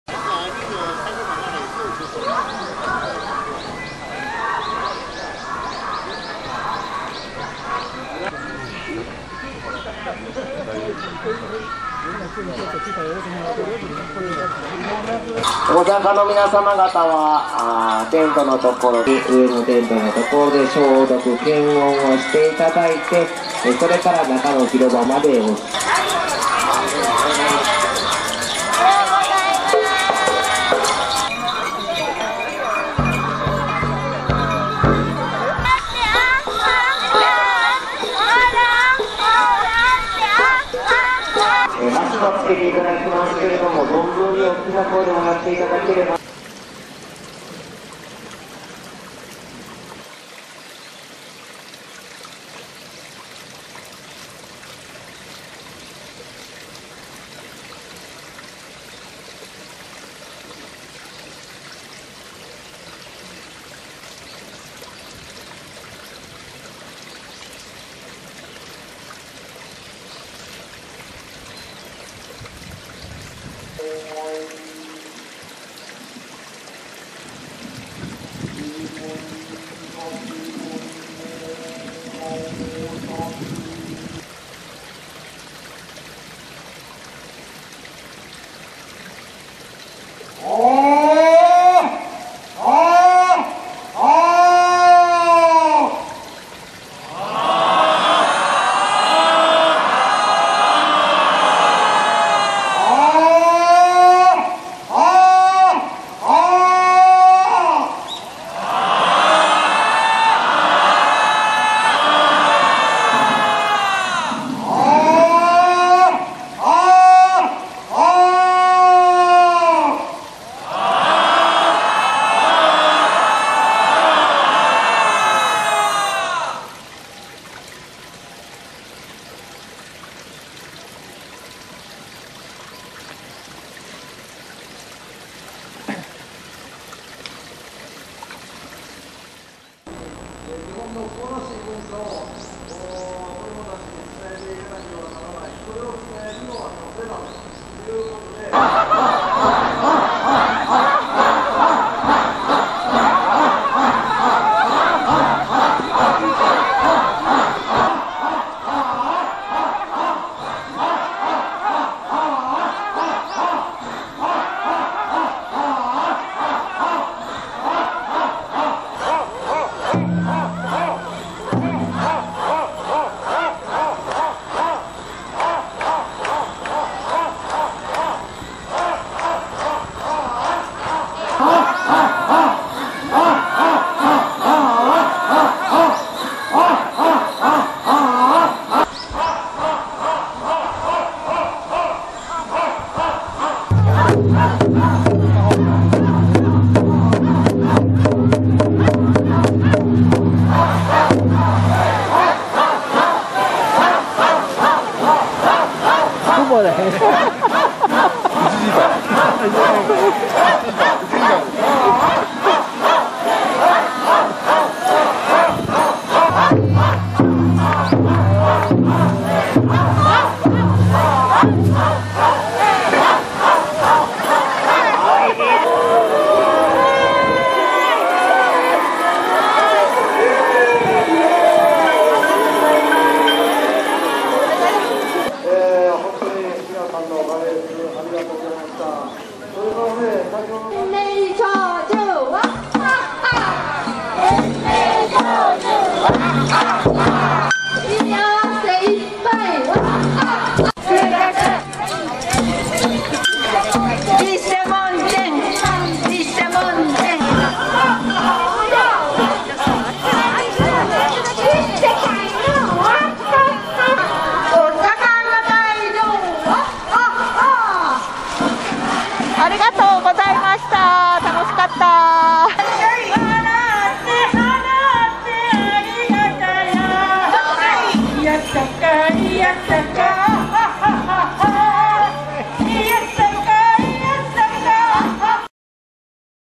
お笑い神事 枚岡神社 2022 令和４
東大阪の枚岡神社では、年末に注連縄掛神事（しめかけしんじ）が
おこなわれます。通称お笑い神事といわれ、宮司がアーハツハーと３度
笑い、そのあとのイベントで、参拝者が２０分間笑うという珍しい行事です。